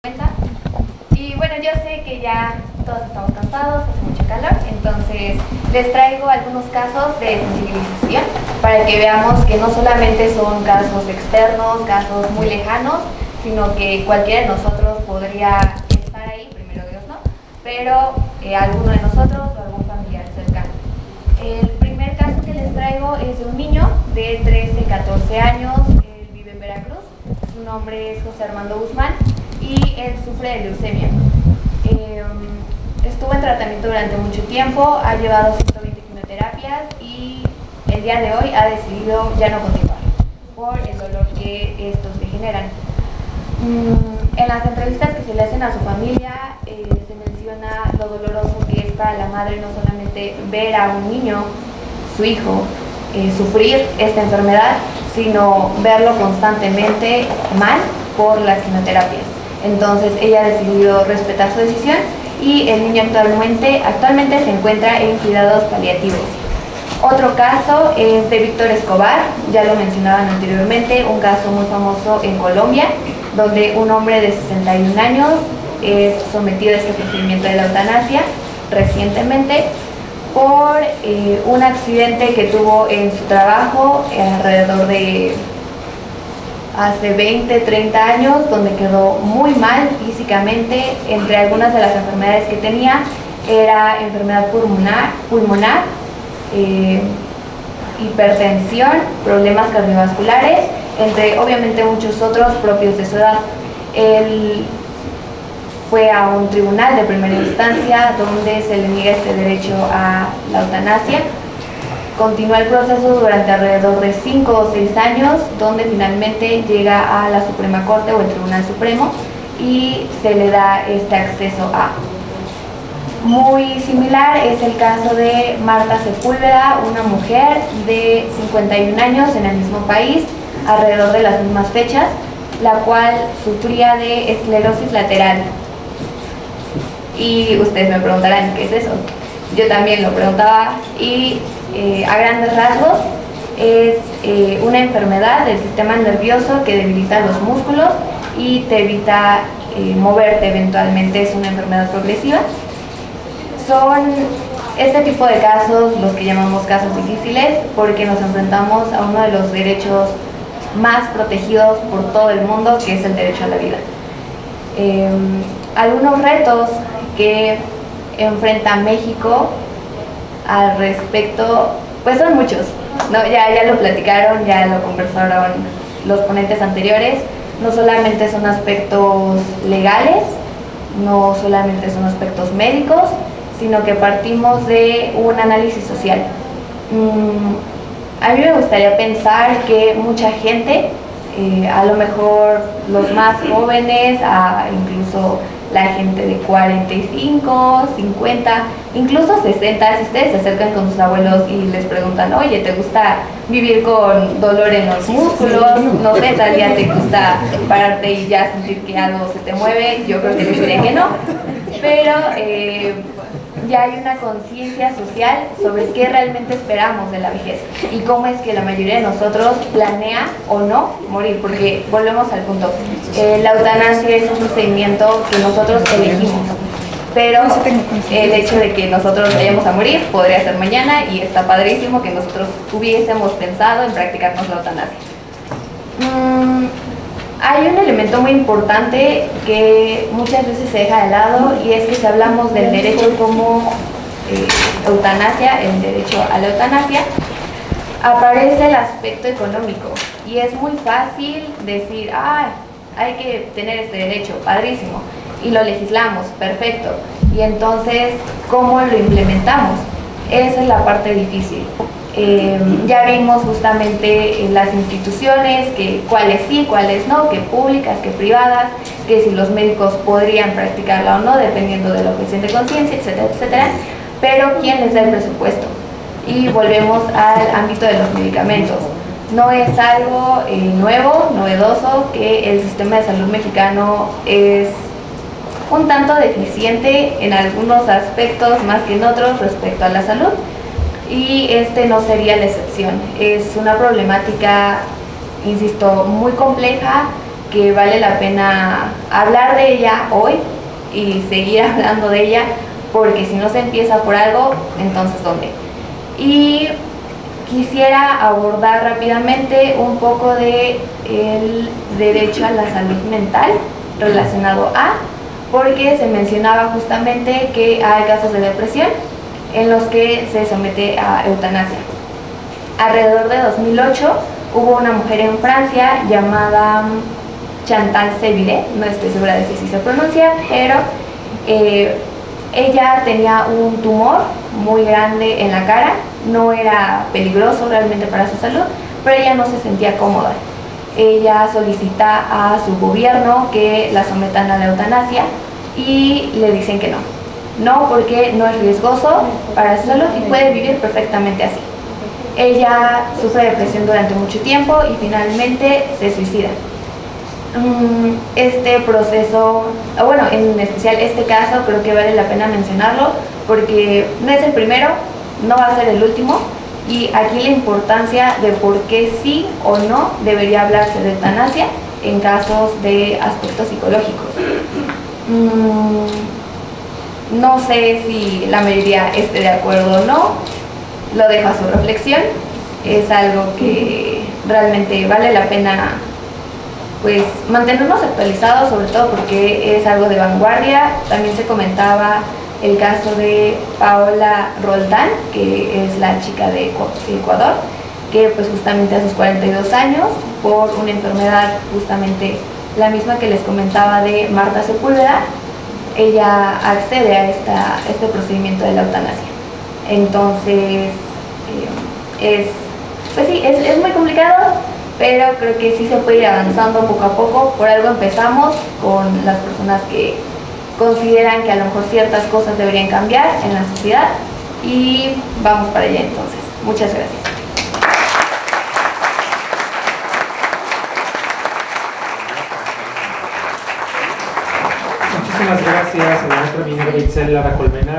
Lic. Minerva Itzel Lara Colmenares
¿EUTANASIA EN MÉXICO?
Mesa de Dialogo
Miércoles 13 de marzo de 2024
11.00 horas
Evento sin costo hibrido,
presencial en el Auditorio Gabino Fraga, Facultad de Derecho UNAM

Convocan:
Facultad de Derecho, a través del Seminario de Filosofía del Derecho
Programa Universitario de Bioética
Programa Universitario de Derechos Humanos

Participantes:
Dra. Jennifer Hincapie Sanchez
Dr. Oscar Enrique Torres
Mtro. Juan Carlos Ruiz Espíndola
Mtro. Rubén Francisco Pérez Sánchez
Moderador:
Lic. Alberto J. Montero

Objetivo:
Analizar la Eutanasia, desde diversas perspectivas, analizando su trayectoria en derecho comparado, sus implicaciones bioéticas y en derechos humanos, así como las perspectivas para su regulación en México

Coordinación:
Alberto J. Montero
Imael Eslava Pérez
Rubén F. Pérez Sánchez